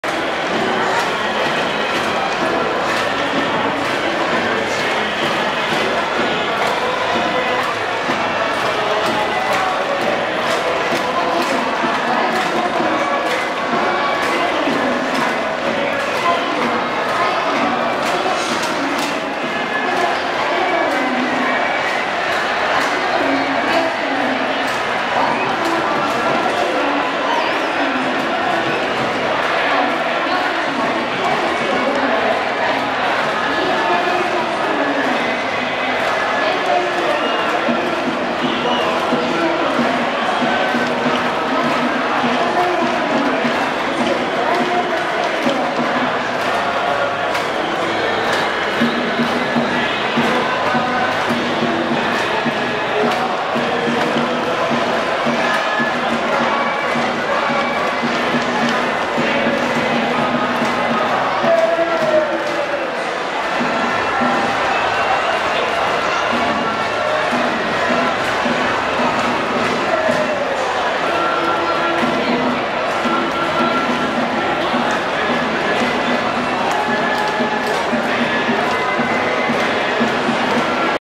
2012_06/29.マツダスタジアム、前田健太、通算５０勝ウイニング・ラン！
（通算５０勝目は）「早くはない。遅いと思う。そこまで意識している数字ではない。
１００勝も通過点になると思う」

君こそが、破天荒。

2012年6月29日（金曜日）　マツダスタジアム
広島東洋カープ 対 横浜ベイスターズ ７回戦

星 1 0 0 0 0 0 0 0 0 = 1 x高崎
鯉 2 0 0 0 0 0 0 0 x = 2 o 前田健太８勝目、今村、sミコライオ

勝利後のお立ち台もおわって、スラィリーとライトへ。
そのあとは１塁側の内野席最前列とハイタッチ！